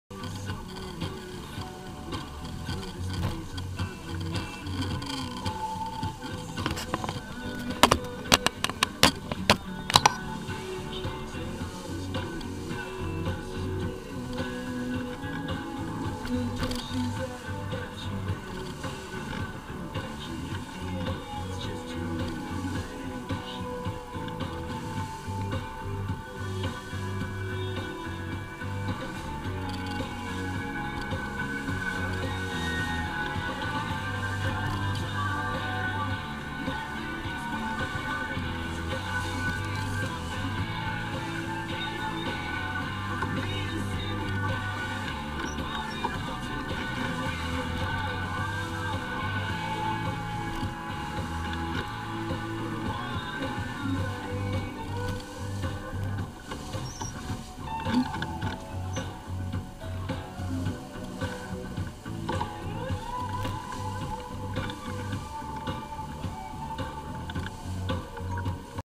Trolling for Fish
Deep Sea Fishing on the Gulf Coast. We were off the Coast of Panama City, Florida.